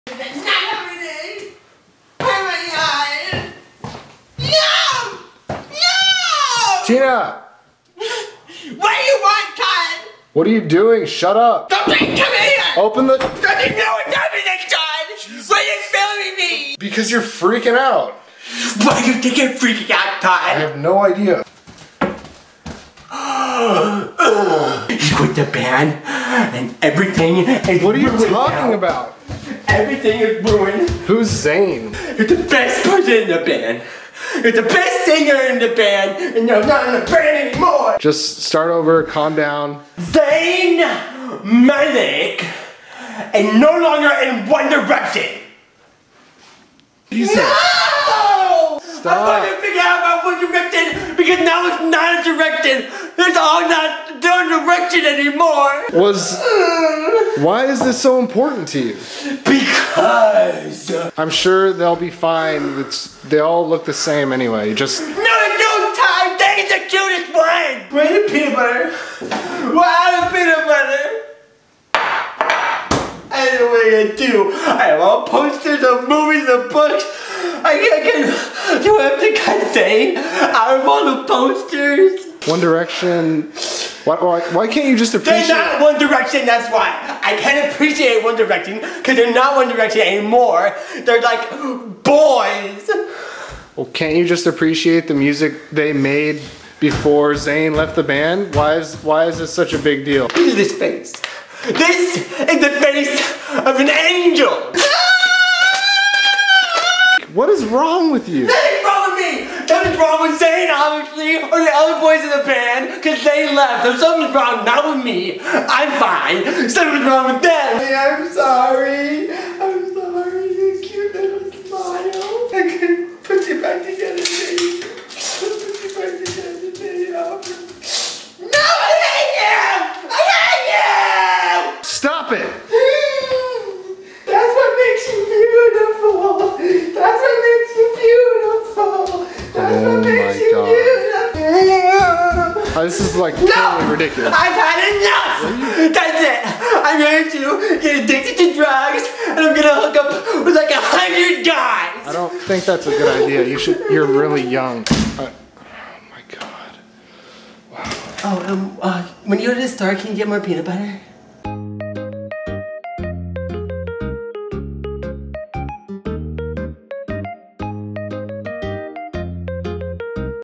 ZAYN QUITS ONE DIRECTION REACTION!
Yesterday my sister started freaking out about some boy band so I filmed it.